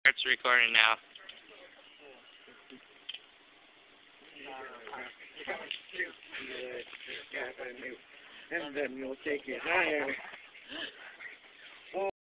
Gasmask song
Two of my friend sing some song while dancing and where gas maskes...